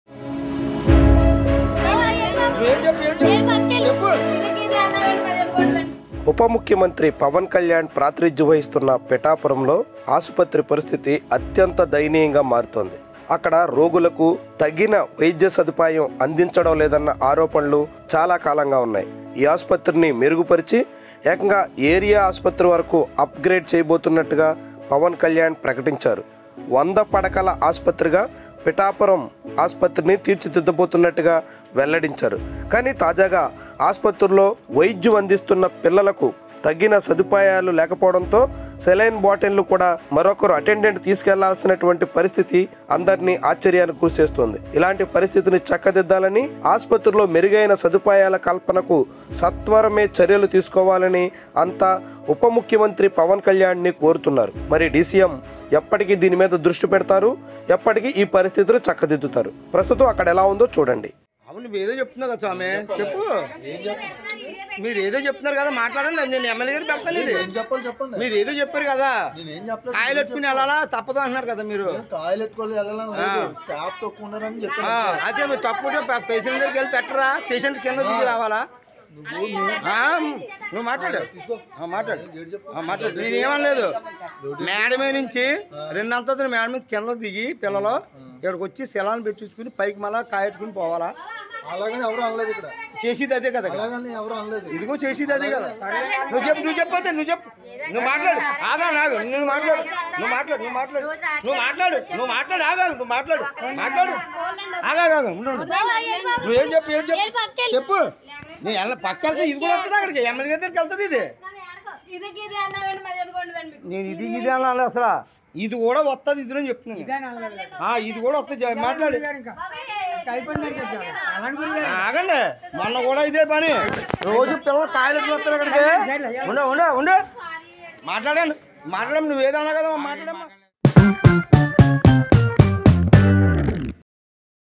Pithapuram: ప్రభుత్వాసుపత్రి దుస్థితి ఎంత దారుణమో! డీసీఎం పవన్ కళ్యాణ్ స్పందిస్తారా?
ఆంధ్రప్రదేశ్ ఉపముఖ్యమంత్రి పవన్ కళ్యాణ్ ప్రాతినిథ్యం వహిస్తున్న పిఠాపురం పరిస్థితి దయనీయంగా ఉంది. చివరకు సెలైన్ ఎక్కించే అవకాశం కూడా లేకపోవడంతో పేషెంట్ అటెండర్ చేత్తో పట్టుకుని తిరగాల్సి వస్తోంది.